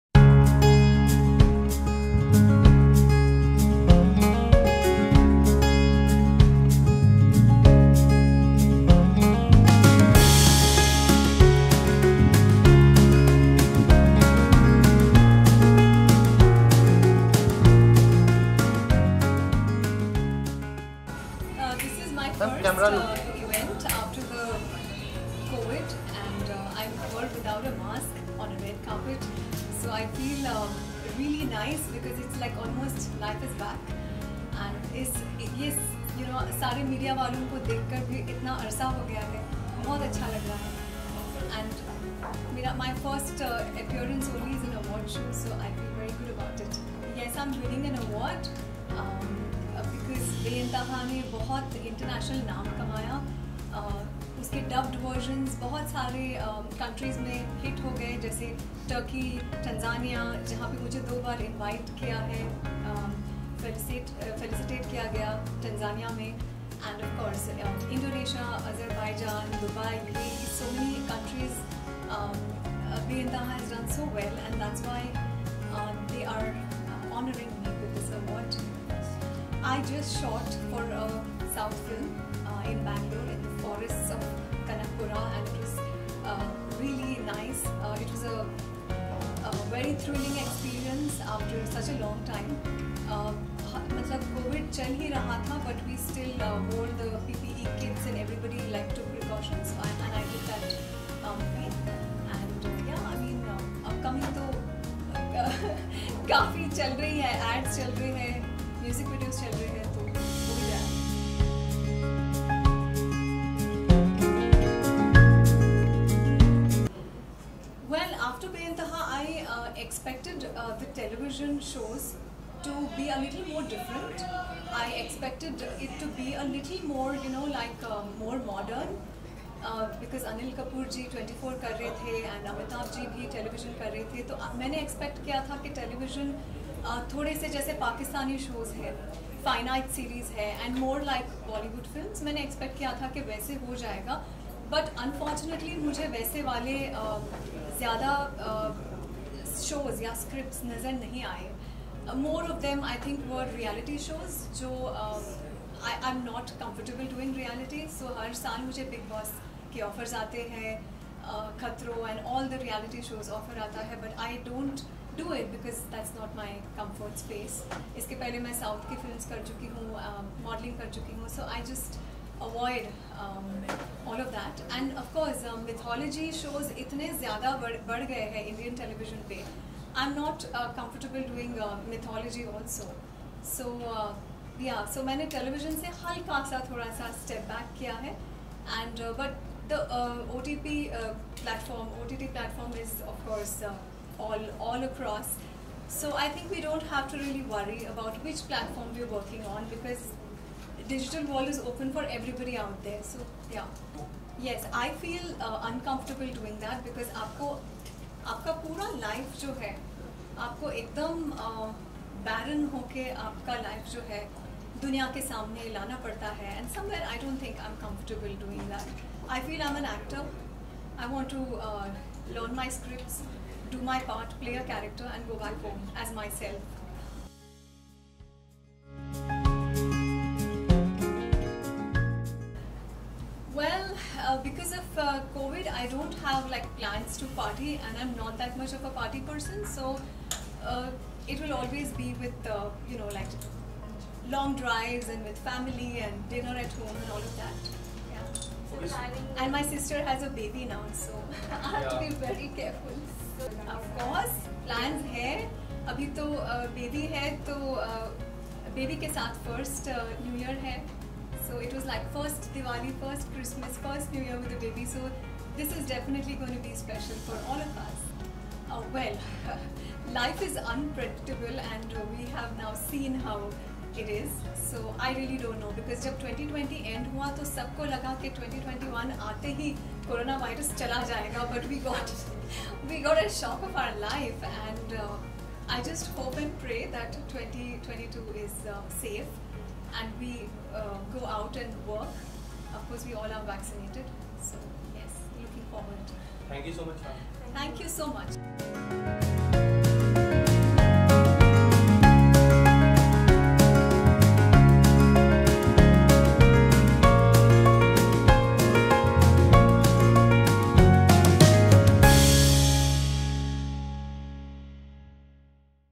Preetika Rao on Amrita Rao's baby boy Veer, Beintehaa, upcoming projects & more
In an interview with Preetika Rao, she tells us about her first event after pandemic without the mask. She feels elated to see the media finally on the red carpet.  This is her first appearance in award show. She is winning an award for 'Beintehaa' which also won repo internationally like Turkey and Tanzania, where she was invited two times, Indonesia, Azerbaijan etc. She recently shot for a south film in Bangalore and felt very happy to get back to work. She expected TV Shows to be different after 'Beintehaa' like fine art series or Pakistani shows but she didn't find it. She also isn't comfortable with reality shows like Big Boss or mythological shows. She has stepped back a little from television. She reveals that she is a family person and doesn't like to party much. She says she wants to take full precaution as her sister recently got blessed with a baby. She looks forward to a great year ahead.